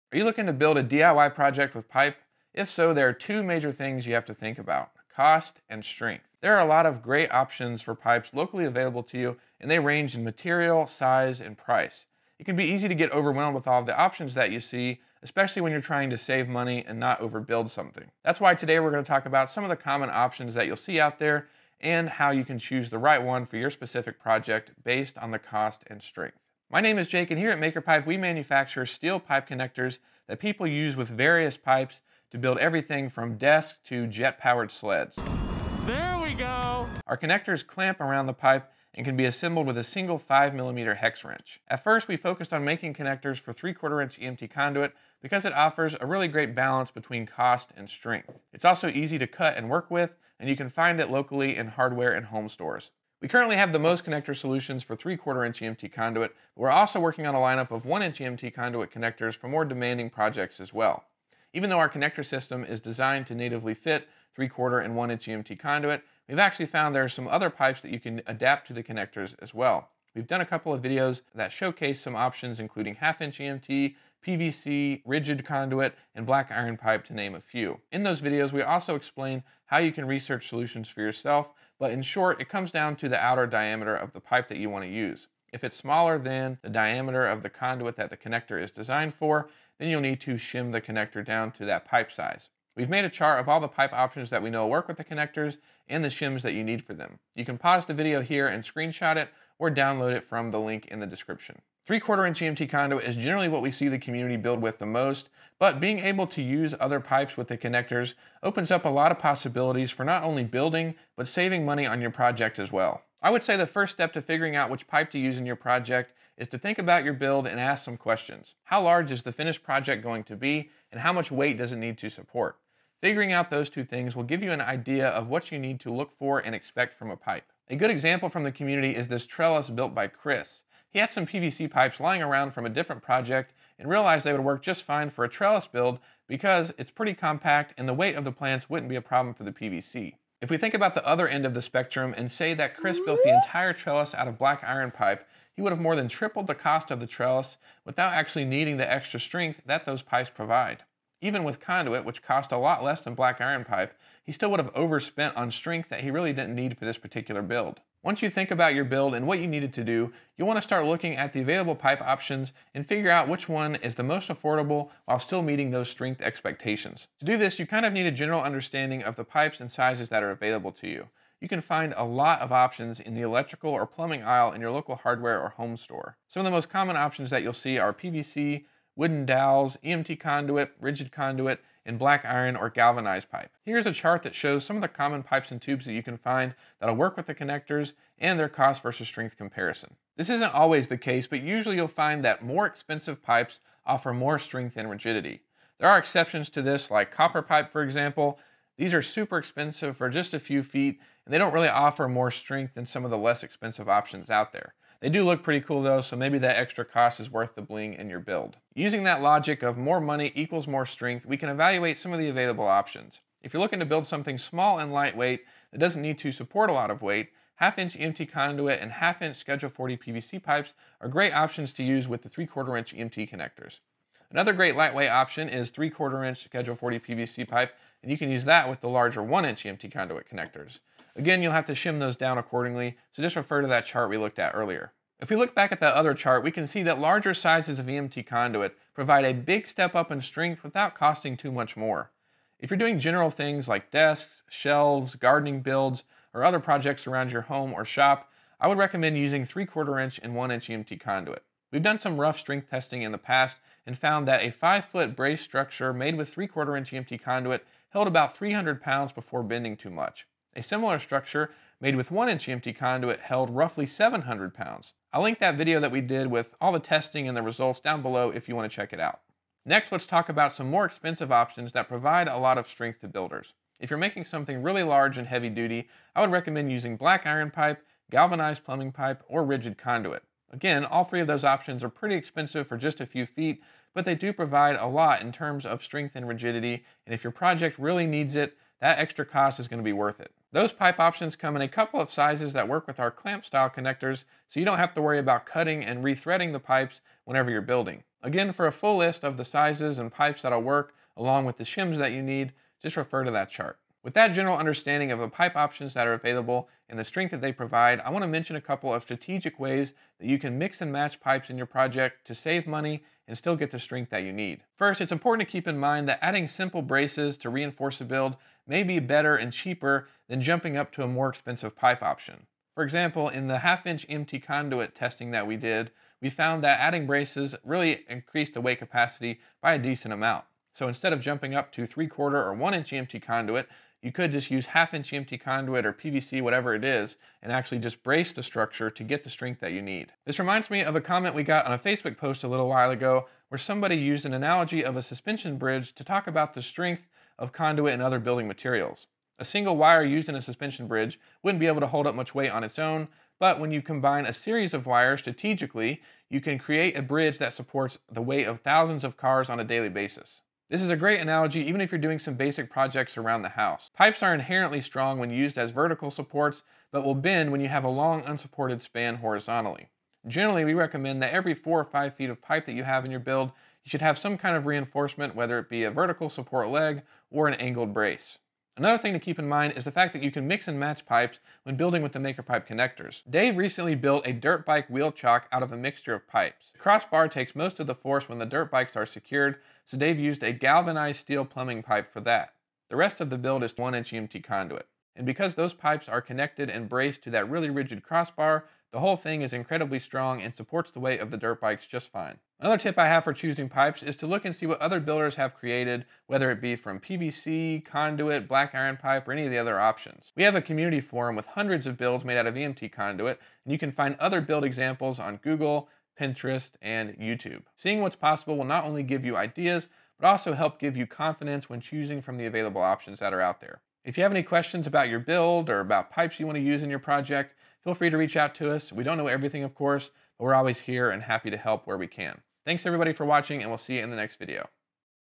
Are You Wasting Money On Your DIY Pipe Projects?
In today's video, we look at some of the common pipes and tubes that we know work with the Maker Pipe Connector System to help make the process of choosing pipes for a DIY project easier. Keep in mind that every project is unique and you always want to do your own testing and evaluation for your builds.